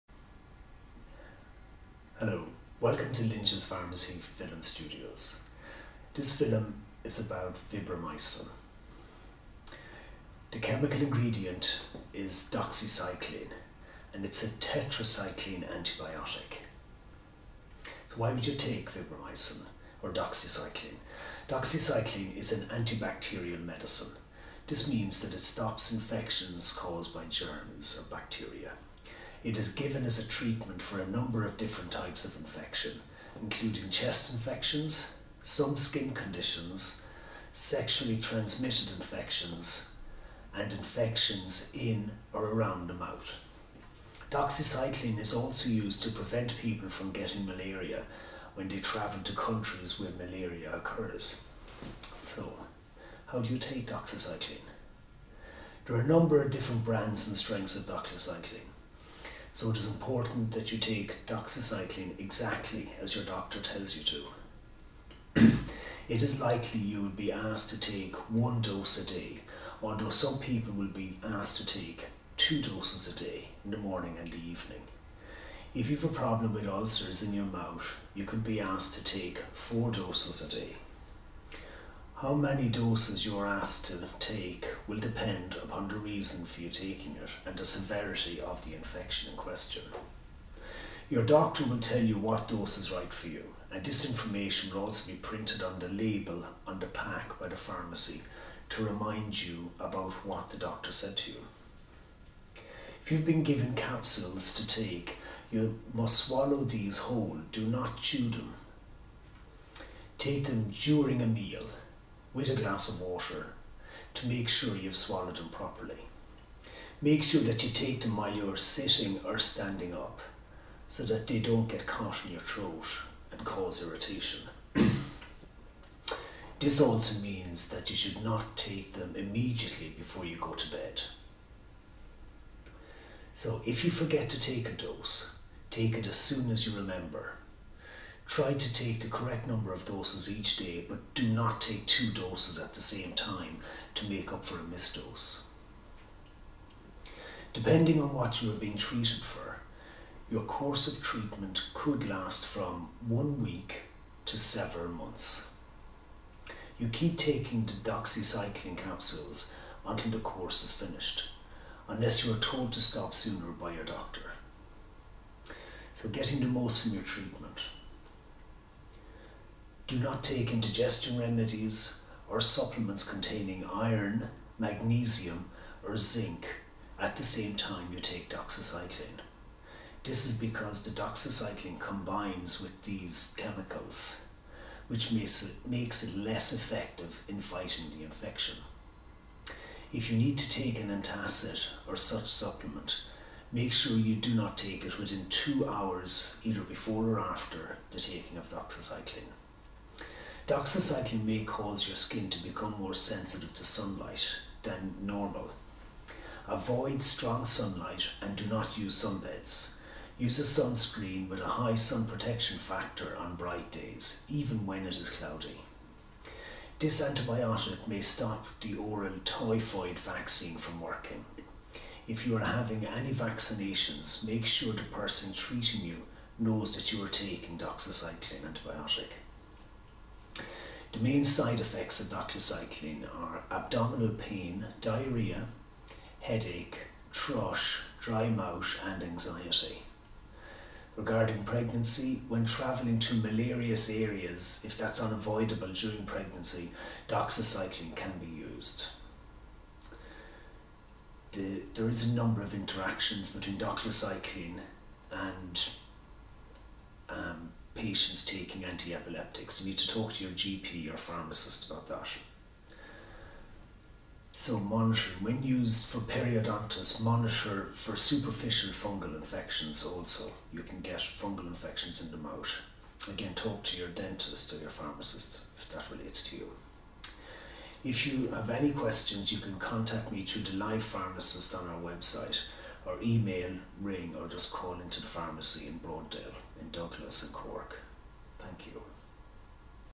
Vibramycin (Doxycycline) antibiotic
Doxycycline is used to treat bacterial infections and is also used to prevent malaria. Other uses include treating pimples or bumps on the face caused by a condition called rosacea, and can be used to treat anthrax infection after possible exposure. This medicine is a tetracycline antibiotic.